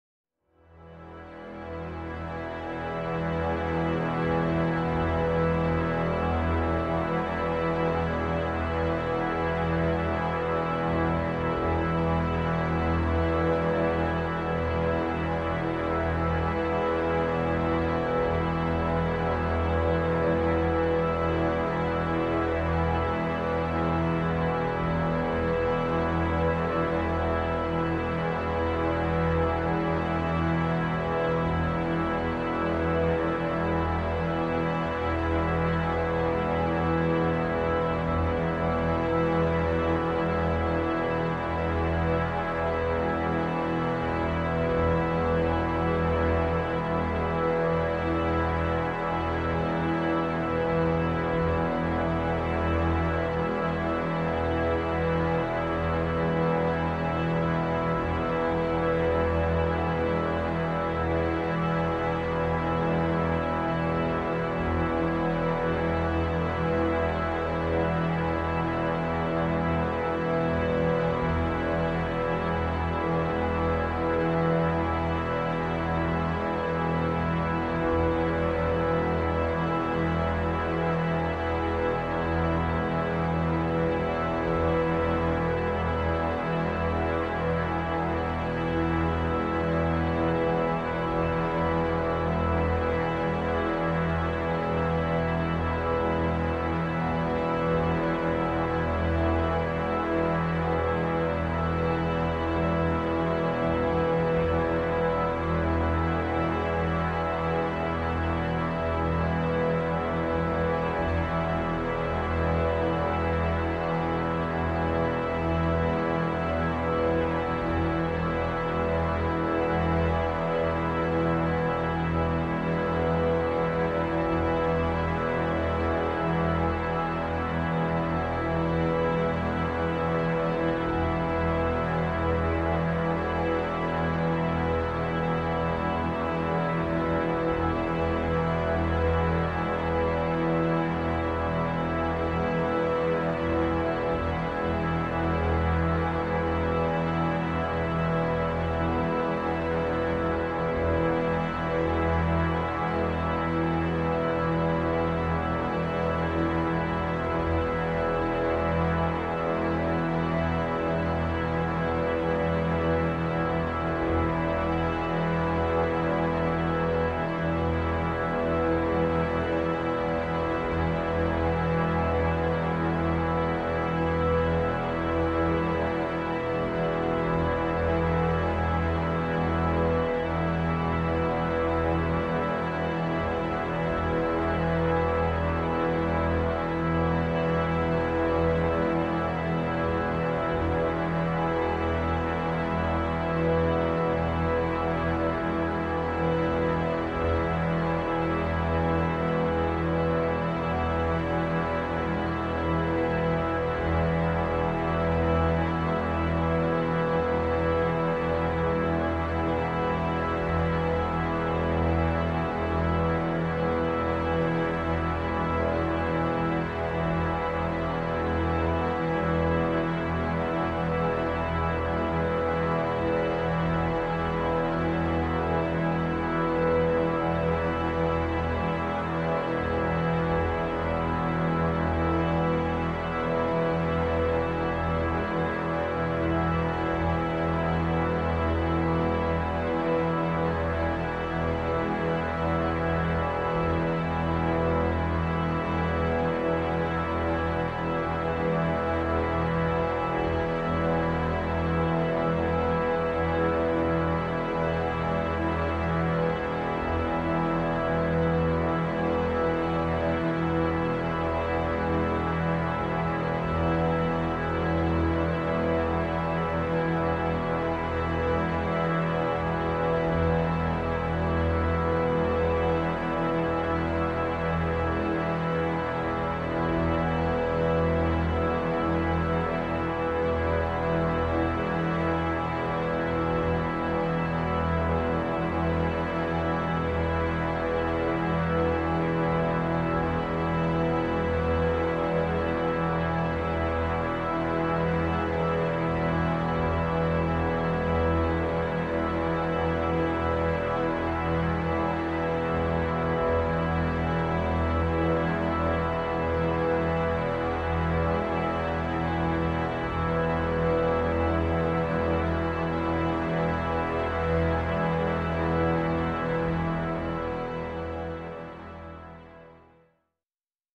E - Orchestra Drone
Practice improvisation and intonation using orchestra winds and strings as your pitch reference.